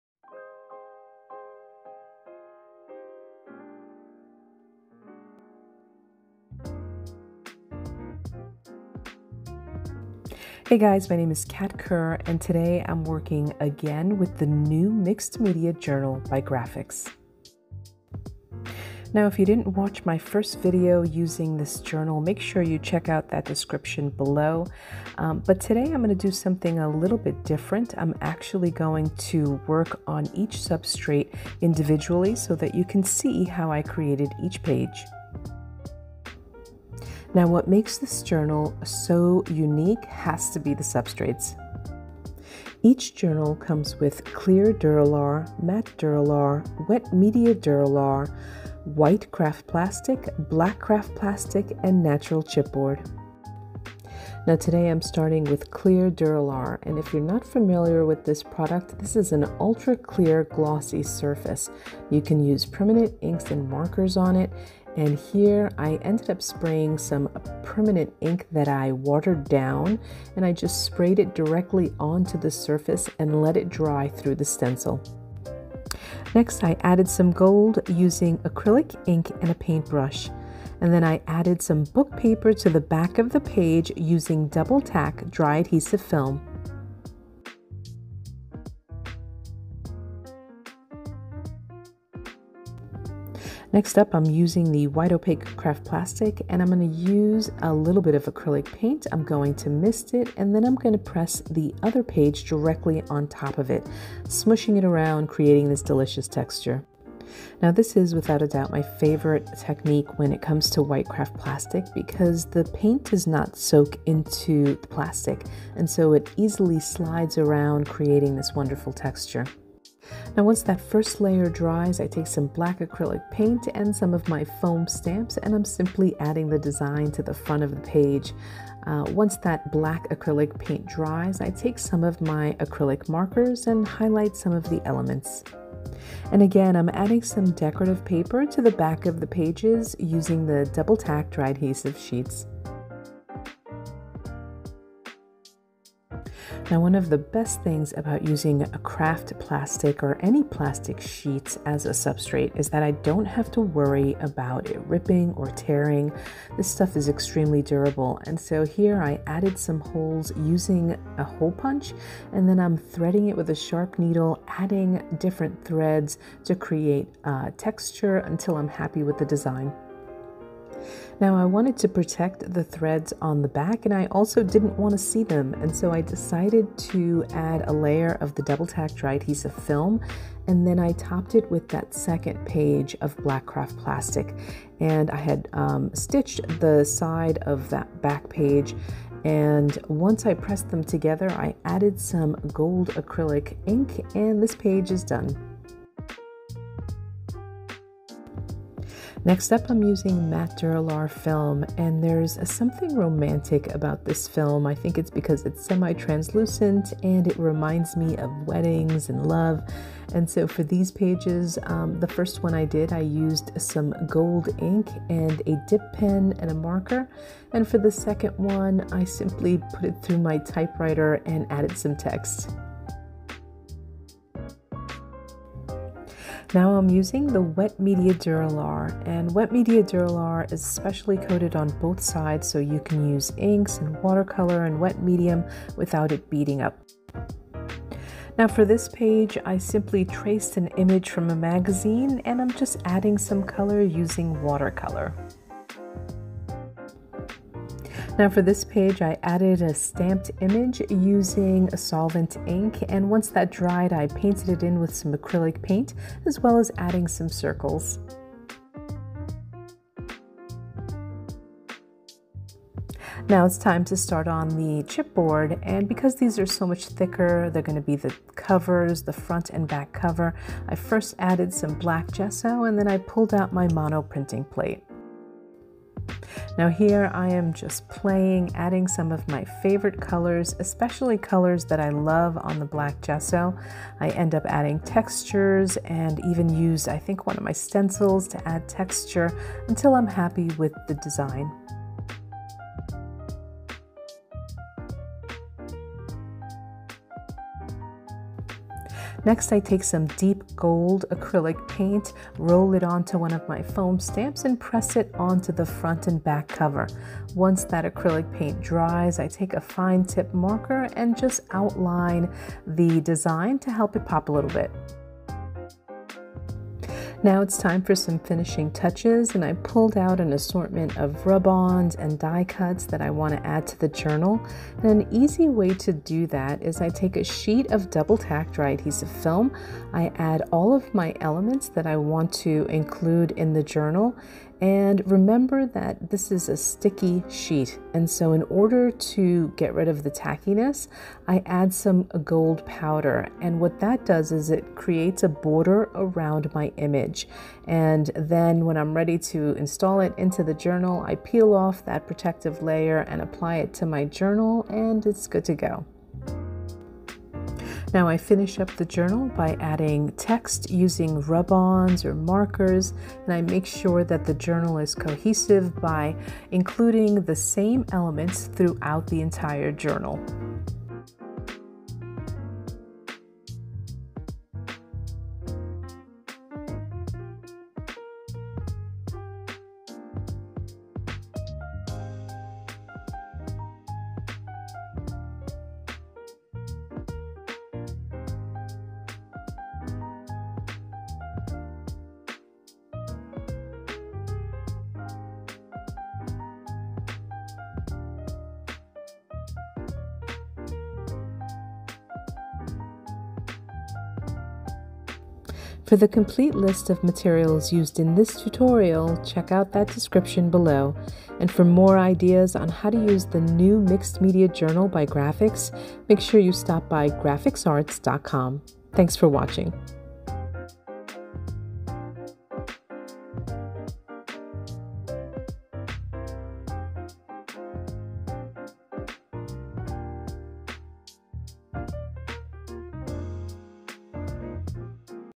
Grafix Mixed Media Journal by Cat Kerr
Open up a journal full of artistic opportunities! Follow along with artist @CatKerrMixedMedia as she creates another bright and fun mixed media journal!

Materials:
Grafix 6 x 6 Mixed Media Journal

Clear Duralar:
Amsterdam Acrylic Inks
Cat Kerr Stencil
Paint Brush
Book Paper
Double Tack Dry Adhesive Film

White Craft Plastic:
Amsterdam Acrylic Paint
Cat Kerr Foam Stamps

Black Craft Plastic:
Tiny hole punch
Thread/String and a large needle
Amsterdam Acrylic Ink

Matte Duralar:
Dip Pen
Amsterdam Acrylic Ink
Amsterdam Acrylic Marker
Typewriter

Wet Media Duralar:
China Pencil
Magazine
Van Gogh Watercolor
Viva Las Vegas/Deedee Catron Heart Stamp
Amsterdam Acrylic Paint

Chipboard:
Black Gesso
Amsterdam Acrylic Paints
Cat Kerr Stencils
Fine Tip Marker

Finishing Touches:
Die Cuts in stash
Gold Mica powder
Rub ons
Text Stickers